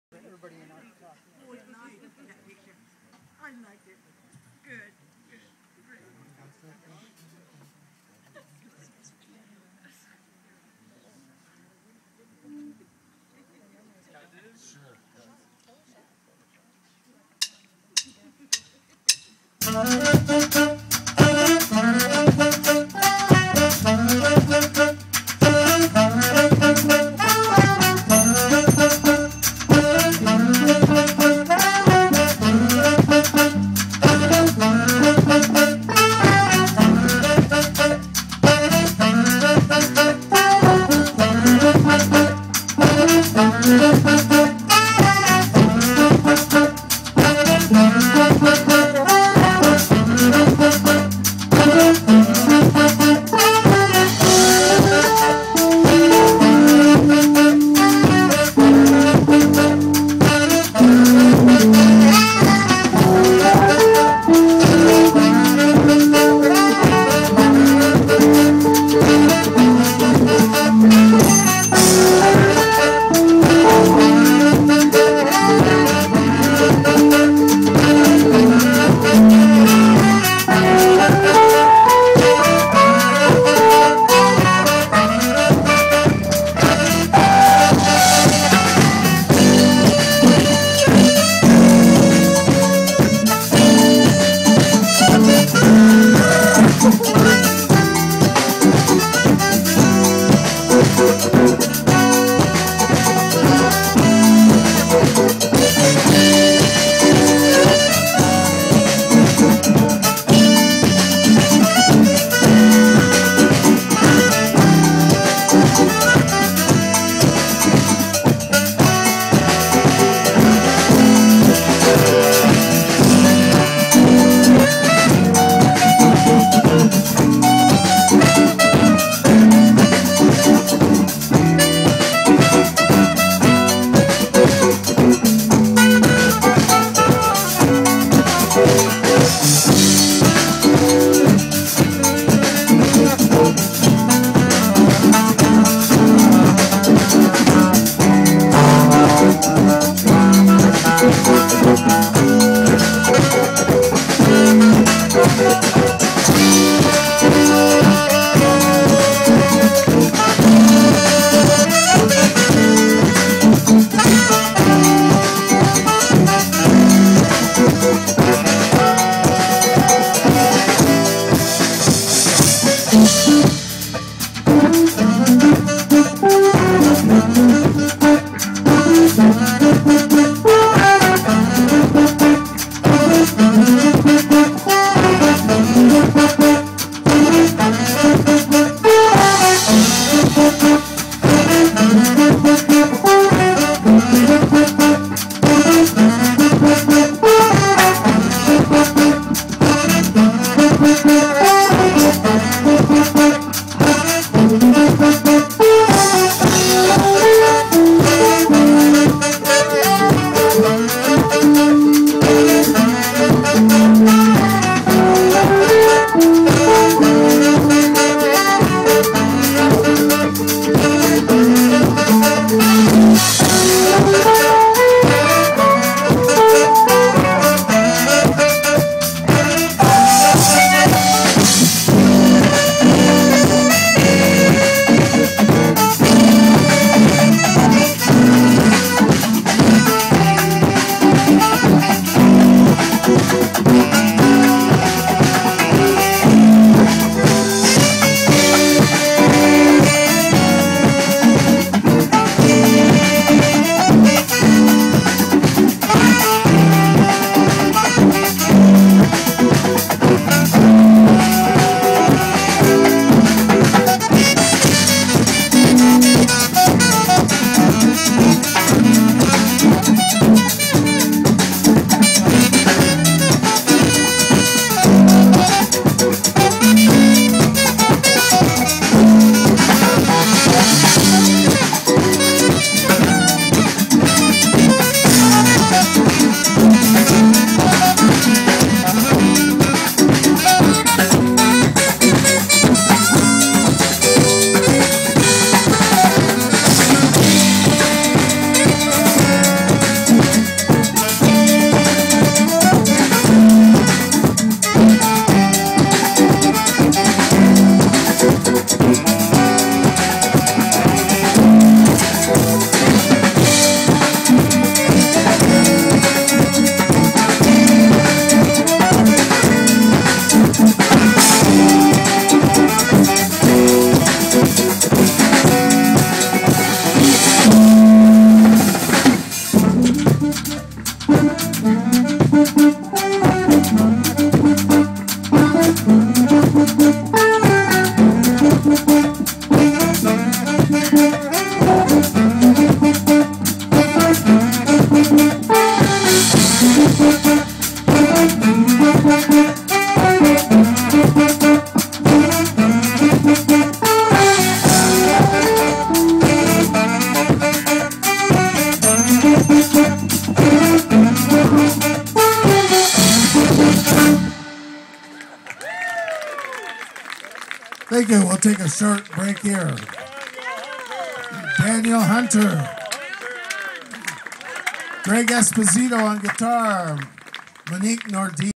Salt Spring Island Jazz Festival 2010...Daniel Hunter.
This amazingly talented 11 year old took the stage as part of the festivals mentor program...he's a natural!